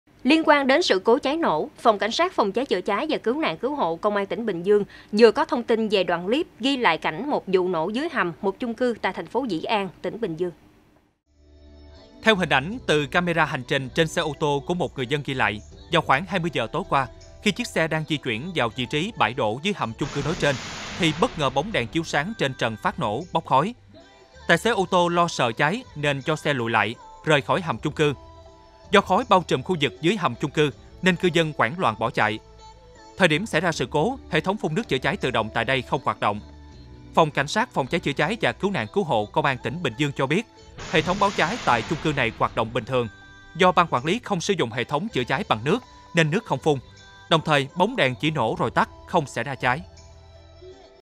Làm rõ vụ nổ trong hầm chung cư tại Bình Dương
Liên quan đến sự cố cháy nổ, Phòng cảnh sát Phòng cháy chữa cháy và cứu nạn cứu hộ, Công an tỉnh Bình Dương vừa có thông tin về đoạn clip ghi lại cảnh một vụ nổ dưới hầm một chung cư tại thành phố Dĩ An, tỉnh Bình Dương.

⬇️⬇️⬇️ Tải THVL Audio để nghe đầy đủ